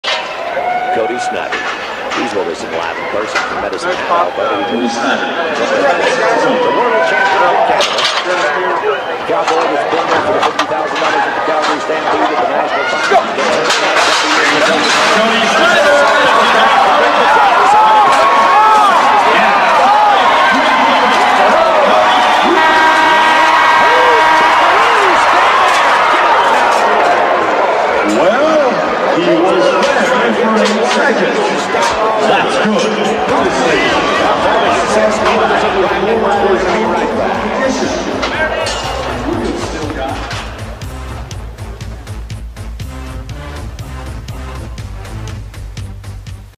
Cody Snyder vs New Boy - 91 BRO Salt Lake (93 pts)
The 2-time PRCA & CPRA Champion bull rider Cody Snyder makes a good ride on Dell Hall's 333 New Boy during a 1991 BRO event in Salt Lake City, UT.